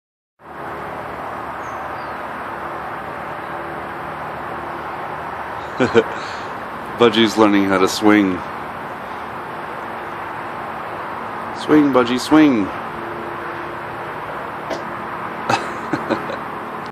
Budgie Swing
Budgie Bird gets excited when I have his seed bowl outside removing the shells. I think he knows he's about to eat!